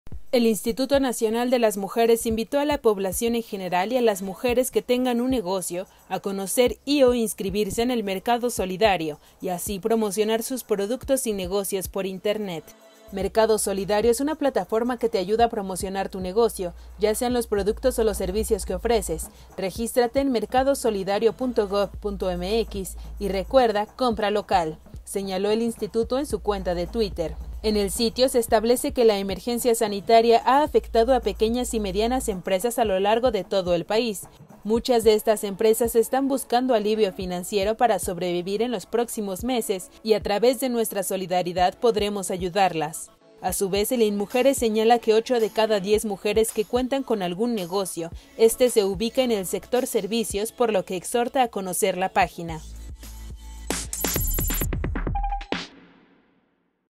Invita Inmujeres a la ciudadanía a conocer el Mercado Solidario
El Instituto Nacional de las Mujeres, invitó a la población en general, y a las mujeres que tengan un negocio a conocer y/o inscribirse en el Mercado Solidario, y así promocionar sus productos y negocios por internet.